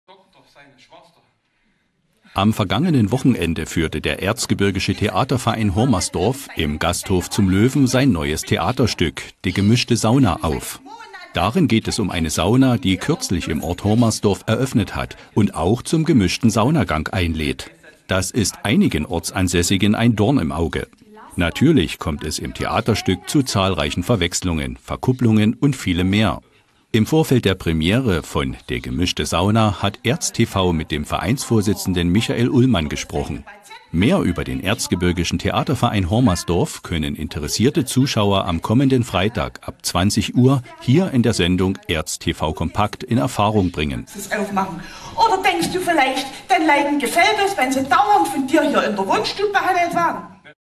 Theaterverein Hormersdorf mit neuem Stück
ZWÖNITZ: Am vergangenen Wochenende führte der Erzgebirgische Theaterverein Hormersdorf im Gasthof "Zum Löwen" sein neues Theaterstück "De gemischte Sauna" auf. Darin geht es um eine Sauna, die kürzlich im Ort Hormersdorf eröffnet hat und auch zum gemischten Saunagang einlädt. Das ist einigen Ortsansässigen ein Dorn im Auge.